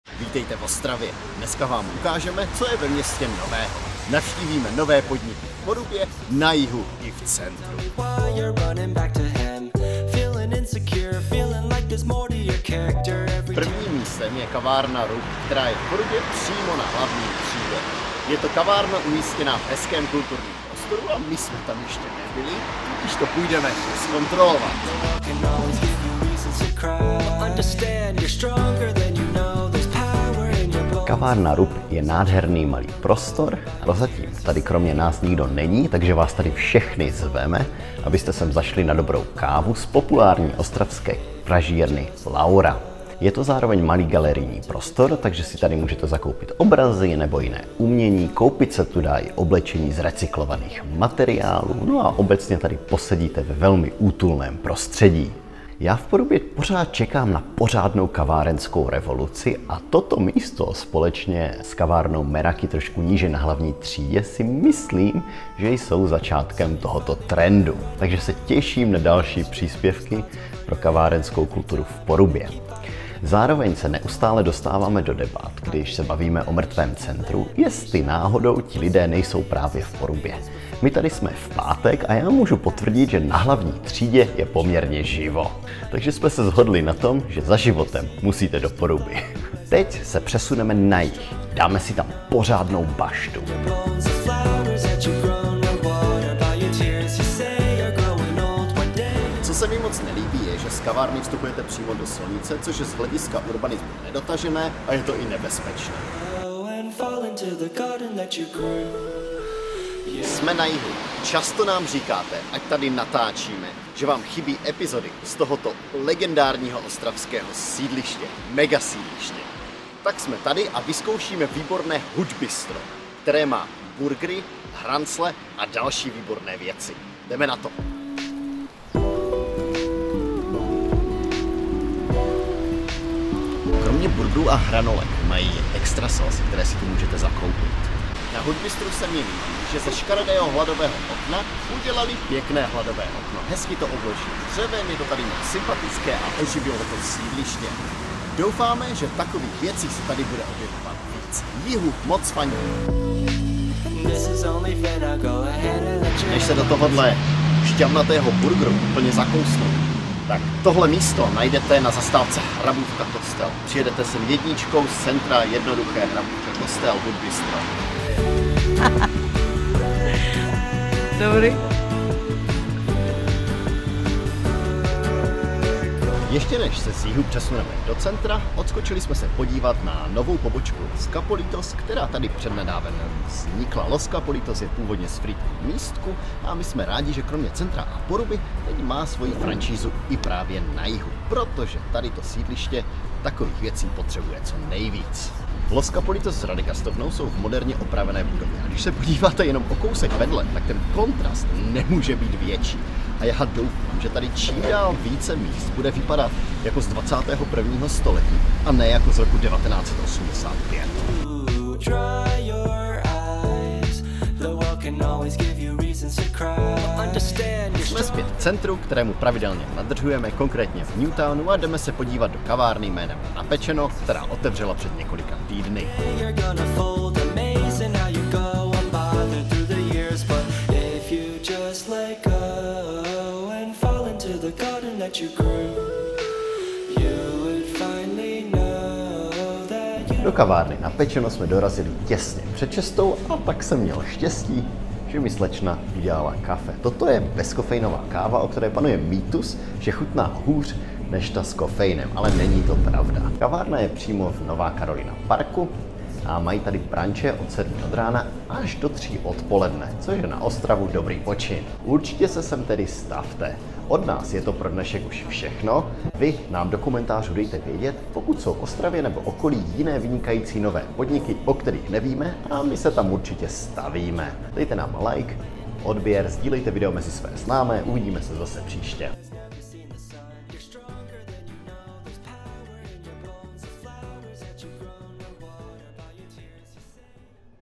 3 NOVINKY V OSTRAVĚ
Poruba, Jih i centrum mají nové cool podniky. Pojďte se do nich s námi podívat!

katerina.petrusova || jan.petrus